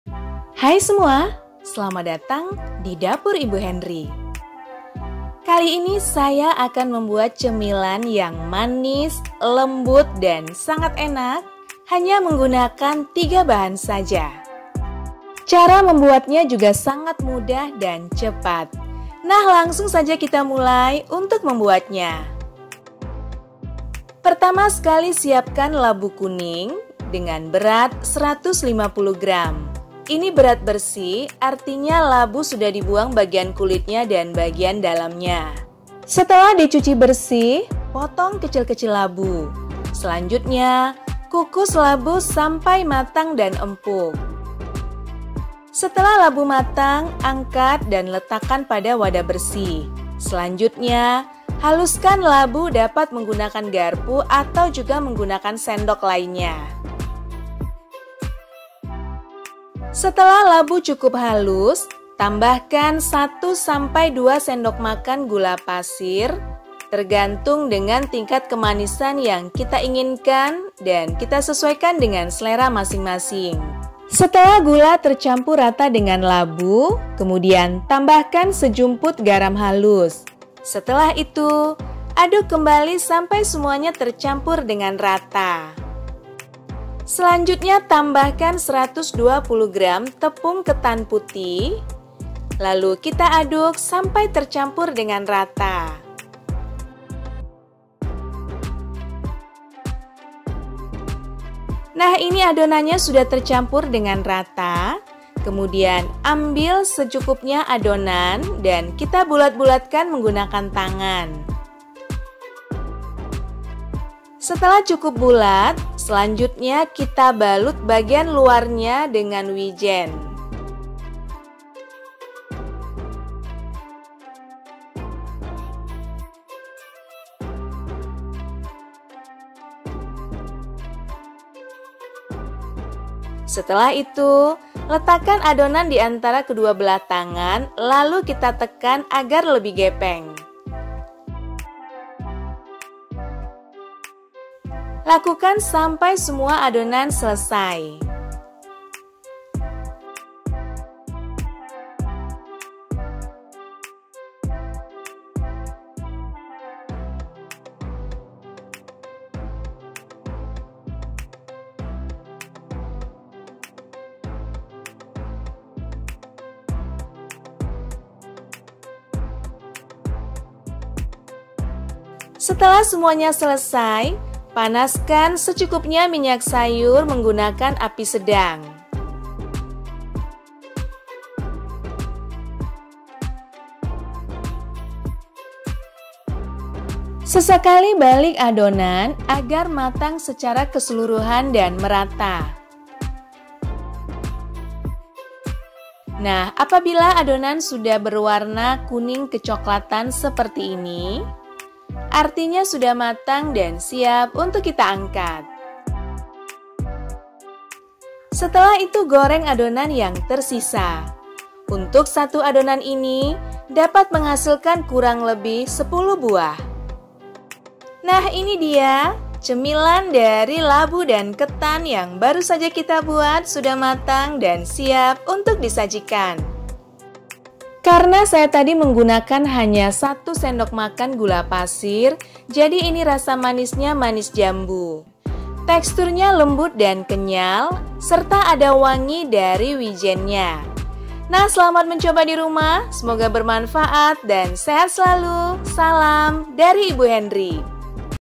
Ketan labu goreng wijen cemilan 3 bahan yang manis & lembut
hai semua
selamat datang di dapur ibu hendri
kali ini sy membuat cemilan dari bahan labu kuning dan ketan putih

bahan
150 gr labu kuning ( berat bersih saat mentah )
120 gr tepung ketan hitam
1 - 2 sdm gula pasir ( 1 sdm manis jambu )
sejumput garam ( optional )